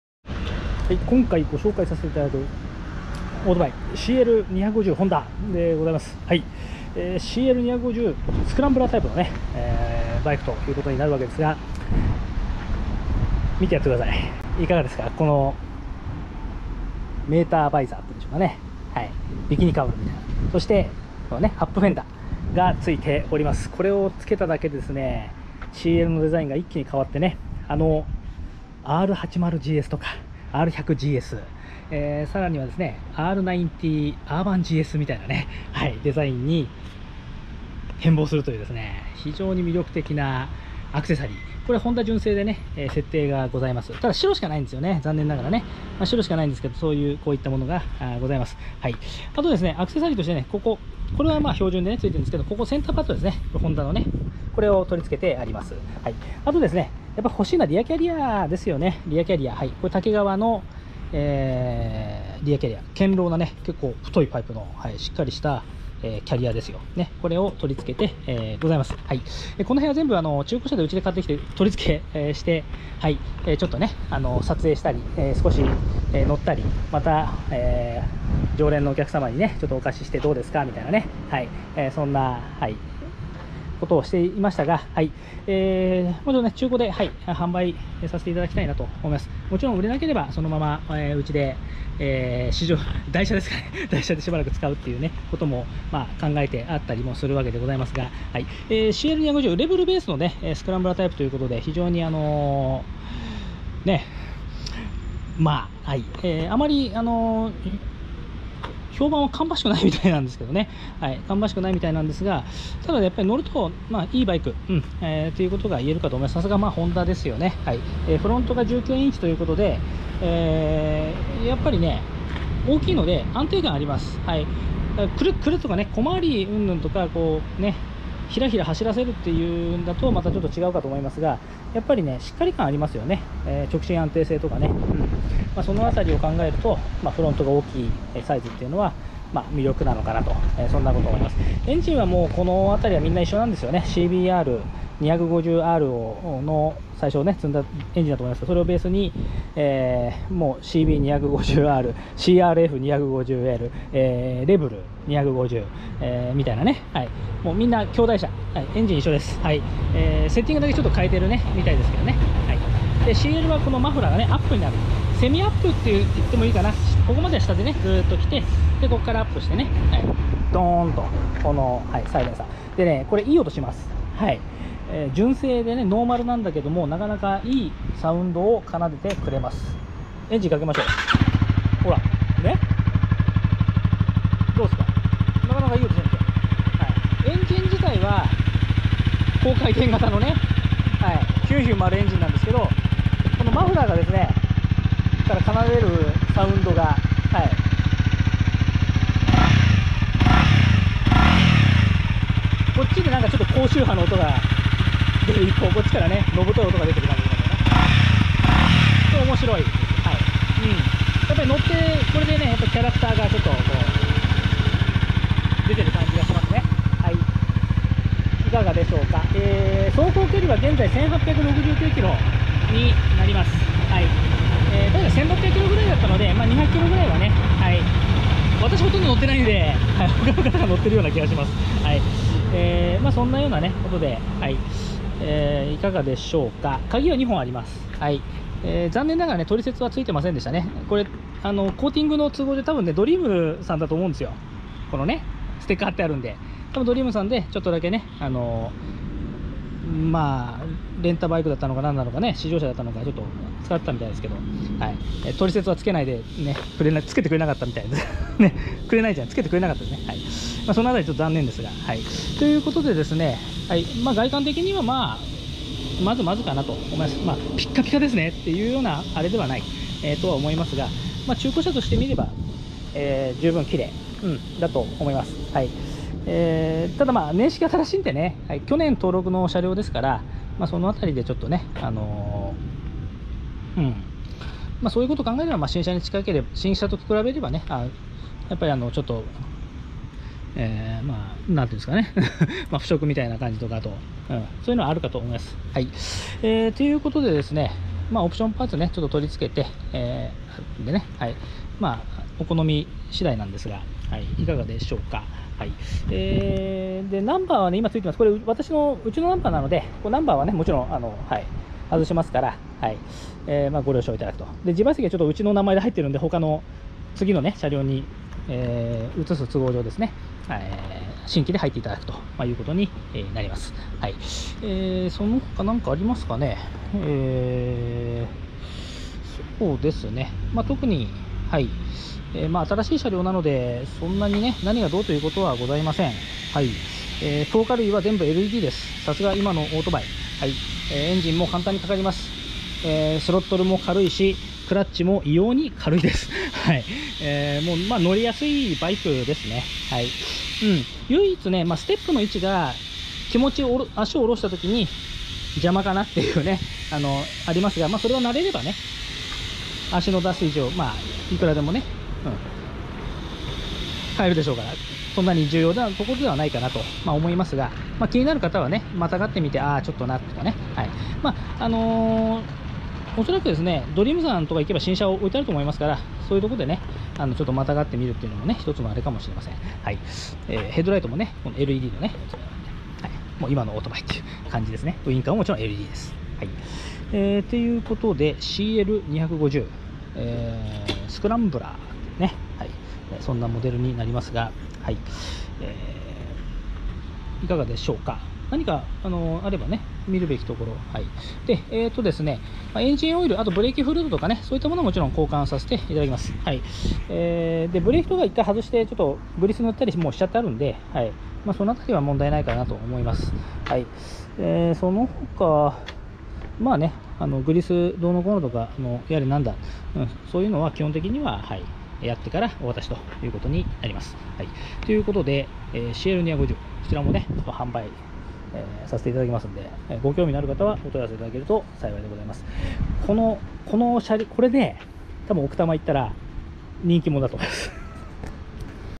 honda CL250 スタイルGS 中古車参考動画
初年度：令和5年5月
メーター距離：1869㎞
保険：なし
鍵：純正2本

純正オプション　メーターバイザー　UPフェンダー　センタータンクパッド
タケガワ　リアキャリア
付

GSにインスパイアされた開発陣が　こんなデザインを
確信犯的なオプションパーツ　つけるとあら不思議どこかのバイクに・・・
でも乗ればそこはホンダ　実にフレンドリー　だれでもその乗りやすさに感心するに違いない